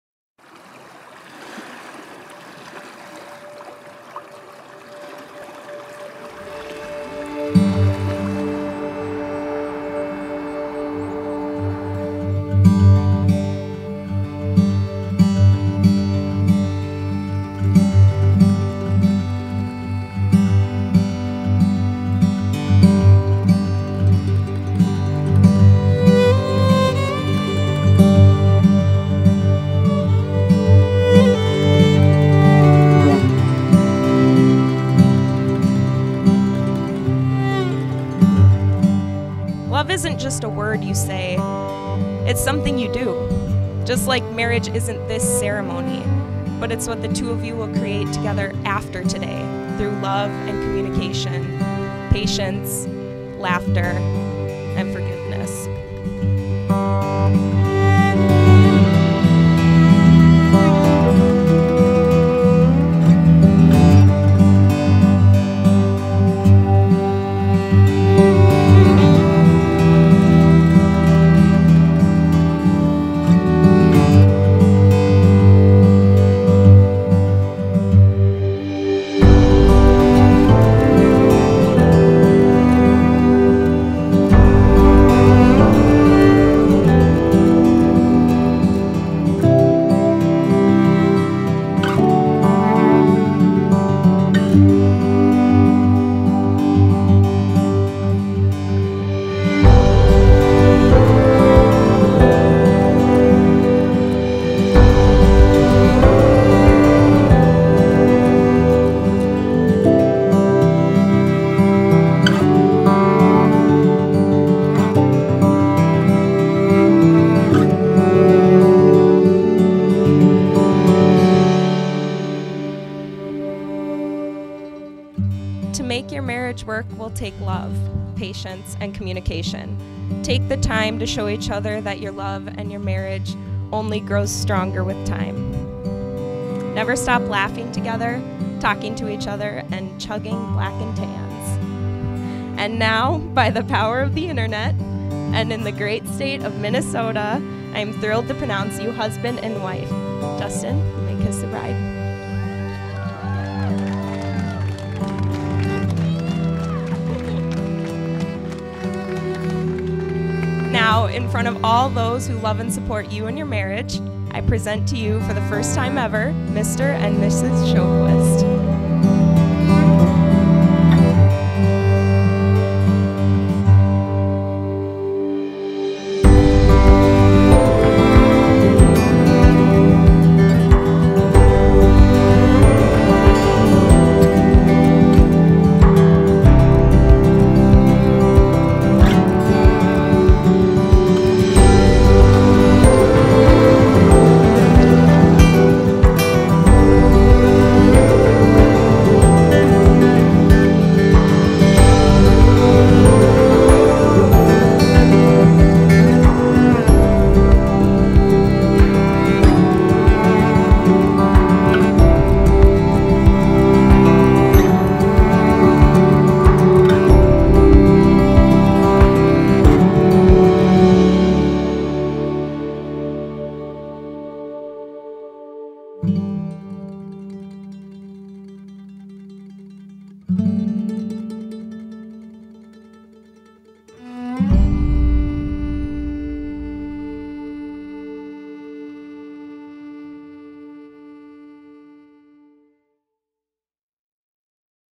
Jenny and Justin Wedding Highlights // 218 Visuals
Can't get enough of this wedding up the shore, I'm so lucky to be a part of amazing days like this one!! Congratulations Jenny and Justin! 🥂🥂